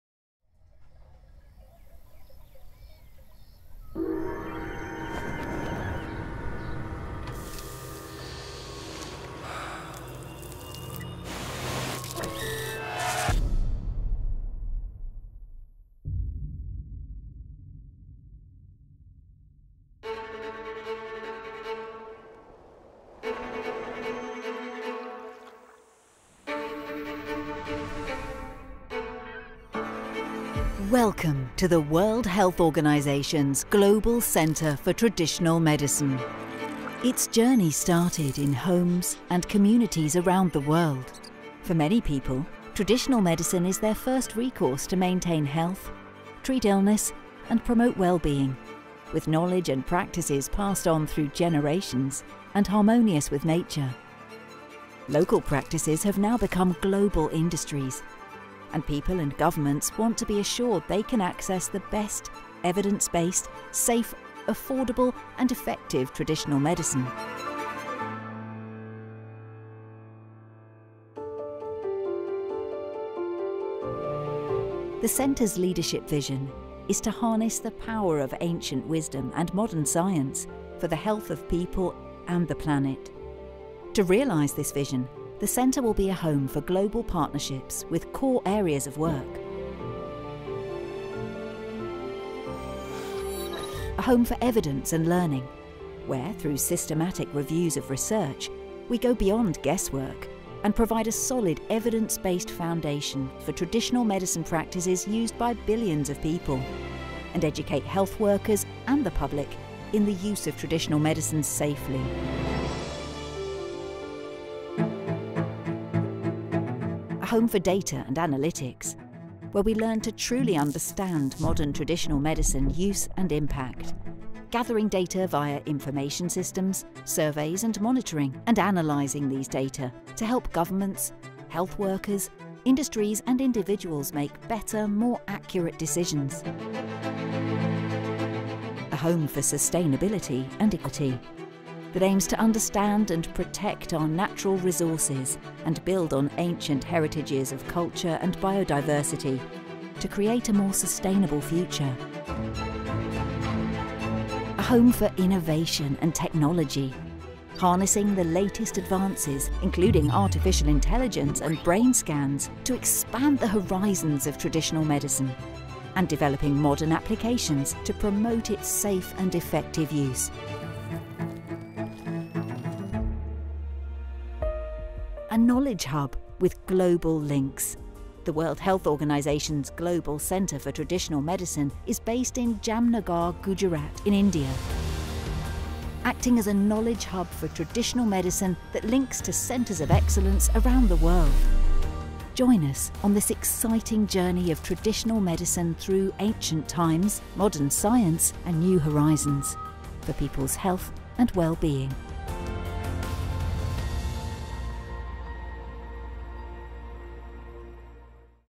Traditional Medicine: five areas of work of the WHO Global Centre for Traditional Medicine (GCTM)
The World Health Organization will collaborate with all countries and WHO regions to focus on evidence and learning, data and analysis, sustainability and equity, and innovation and technology to harness the potential contribution of traditional medicine – supported by modern science and principles of diversity, equity and sustainability – for the health of people and the planet.
The WHO Global Centre for Traditional Medicine (GCTM) will maximize the role of traditional medicine in global health.